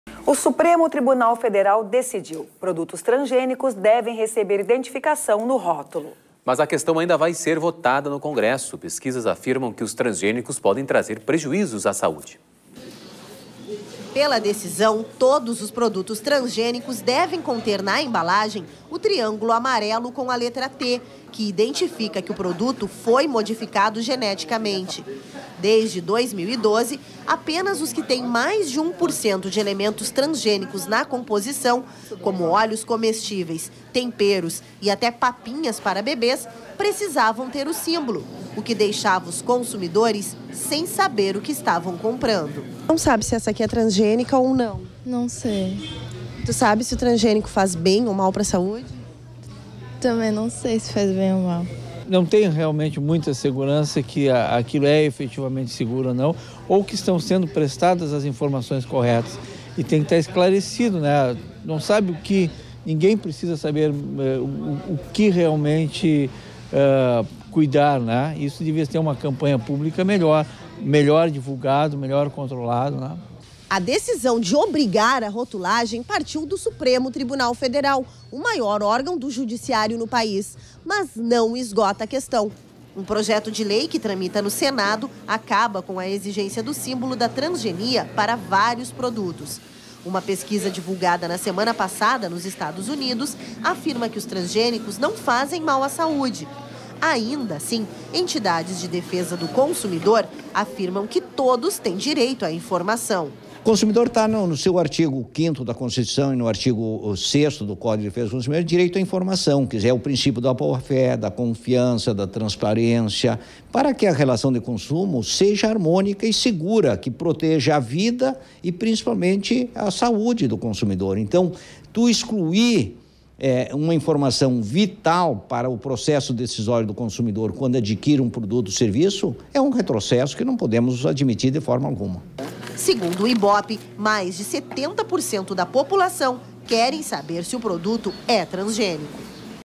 STF quer que transgênicos tenham identificação no rótulo, mas Congresso ainda vai discutir o tema
O Supremo Tribunal Federal decidiu: produtos transgênicos devem receber identificação no rótulo. Mas a questão ainda vai ser votada no congresso. Pesquisas afirmam que os transgênicos podem trazer prejuízos à saude.